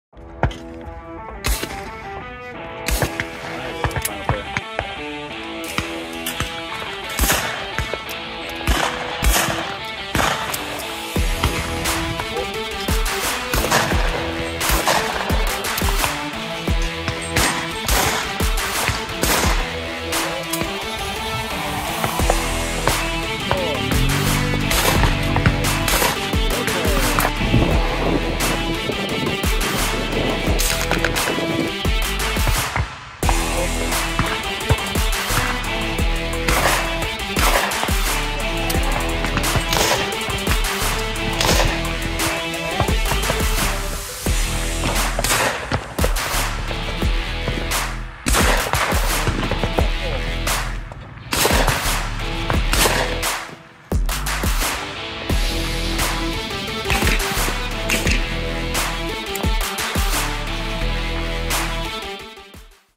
Indian Hills Sports Shooting - 2024 ACUI/SCTP Nationals Highlights
The Warriors took home a national runner-up finish at the 2024 ACUI/SCTP National Championships